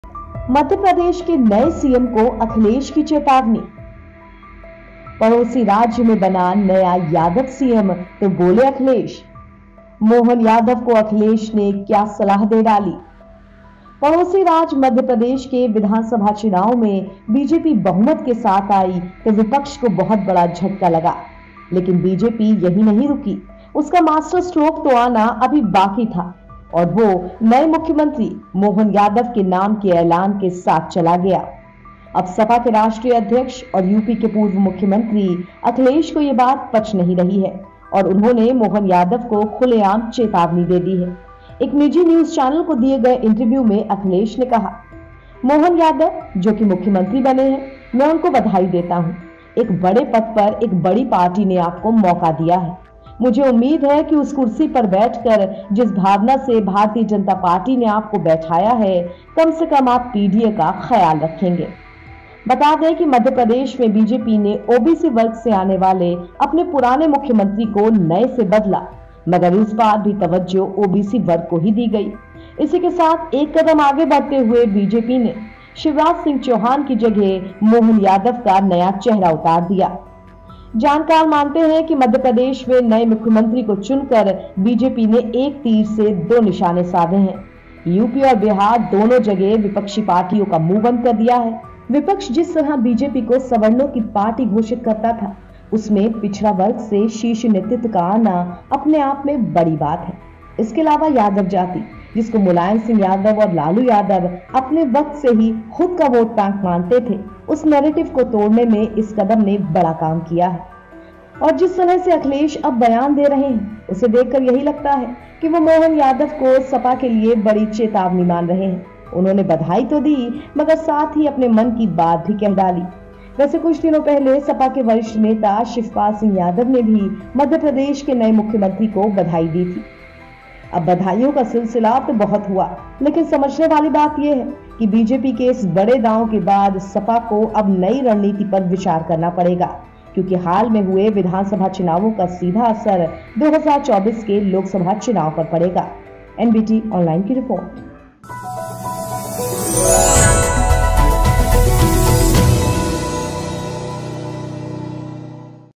Akhilesh Yadav ने दे डाली चेतावनी, MP के नए CM Mohan Yadav को बोल गए ये बात | Lok Sabha Chunav 2024
पड़ोसी राज्य मध्य प्रदेश के विधानसभा चुनाव में बीजेपी बहुमत के साथ आई तो विपक्ष को बहुत बड़ा झटका लगा। लेकिन बीजेपी यहीं नहीं रुकी उसका मास्टर स्क्रोक तो आना अभी बाकी है, और वो नए मुख्यमंत्री मोहन यादव के नाम के ऐलान के साथ चला गया।